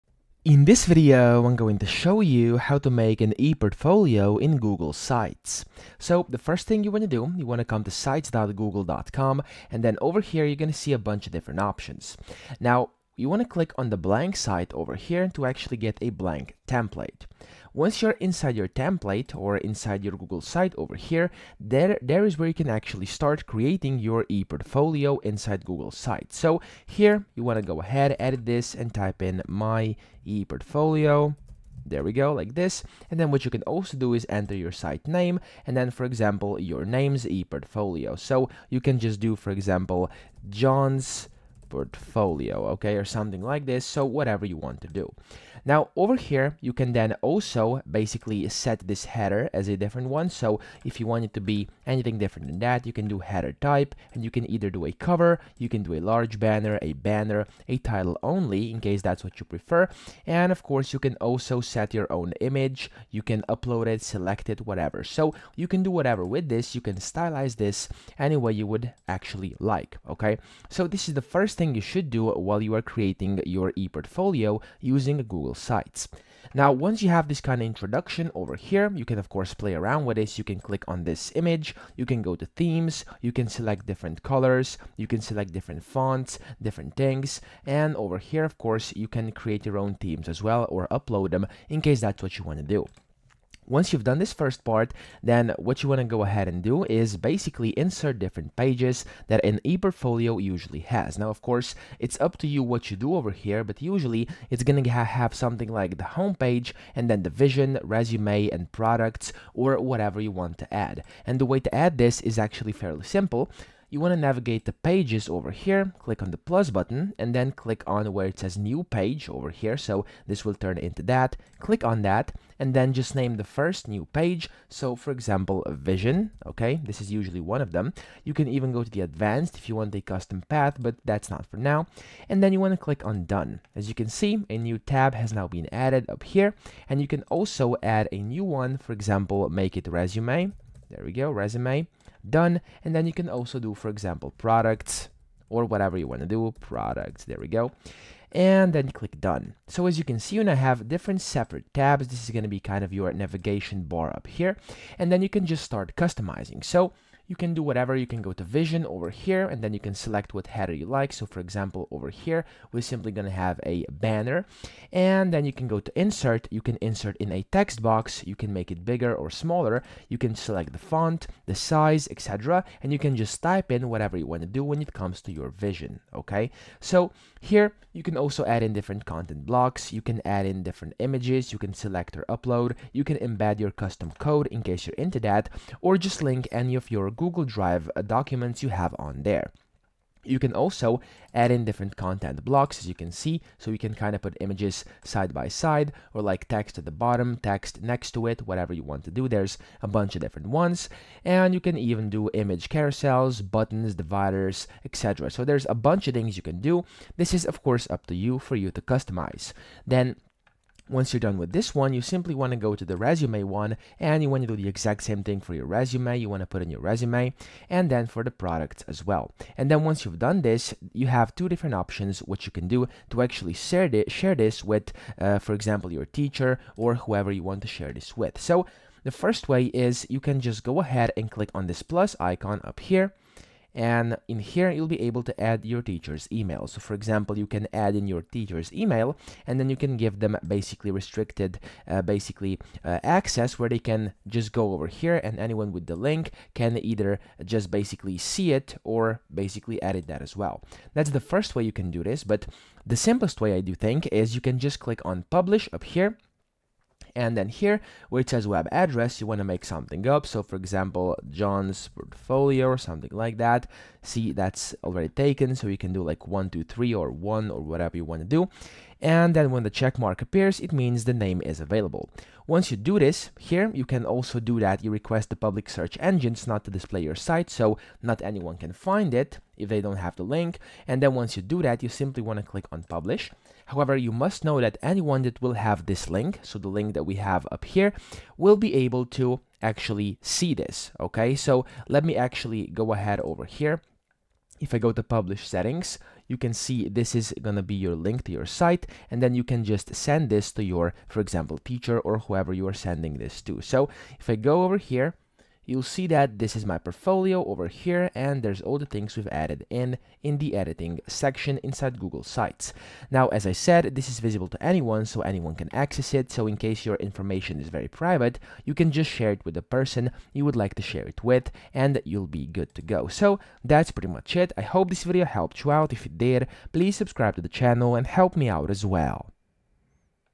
How To MAKE ePortfolio In Google Sites (QUICK & EASY) 2026
In this video, I'll show you how to make an ePortfolio in Google Sites. This is a full guide on how to create, design, and organize a professional ePortfolio using Google Sites in 2026.

In case you've been trying to build an ePortfolio on Google Sites, this is the video you need to see!